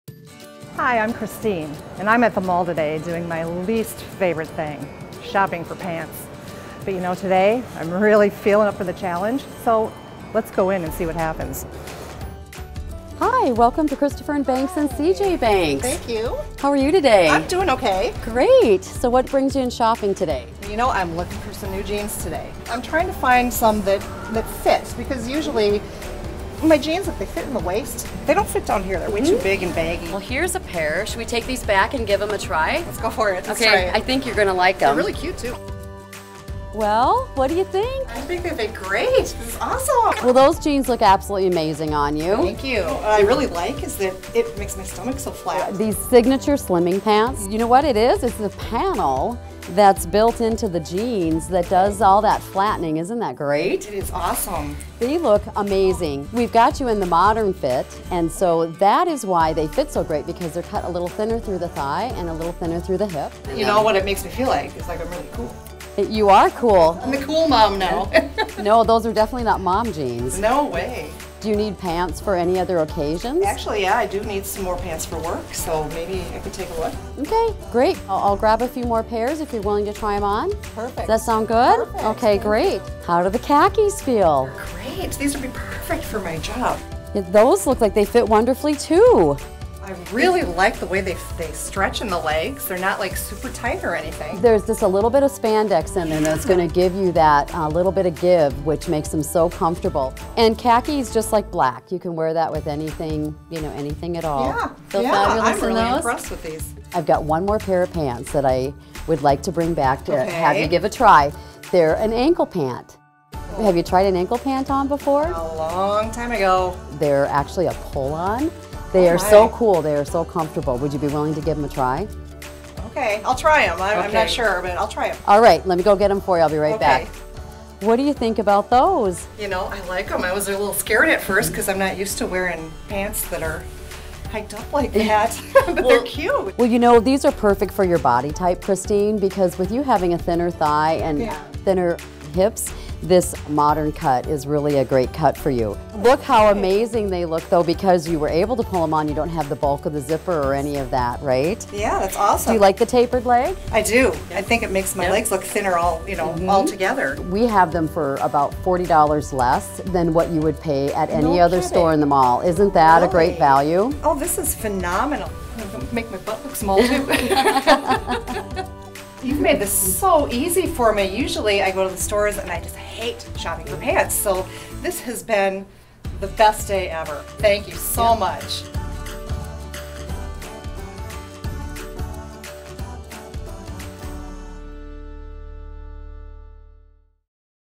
Christopher & Banks / CJ Banks -- Signature Slimming™ Pants and Jeans
Christopher & Banks / CJ Banks Signature Slimming ™ Pants & Jeans -- Maximize your look in our new slenderizing pants. Available online and in-stores.